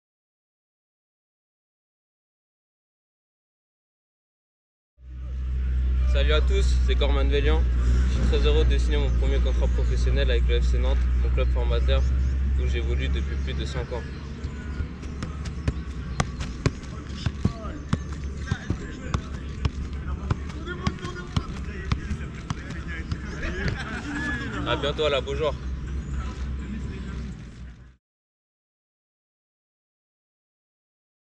Vidéo 360° : Découvrez Gor Manvelyan en quelques jongles
Vivez une expérience en 360° inédite. Faites connaissance à travers quelques jongles avec l'attaquant Gor Manvelyan (19 ans). Le joueur, formé au Club, s’est engagé jusqu’en 2024 sous les couleurs du FC Nantes, son club formateur.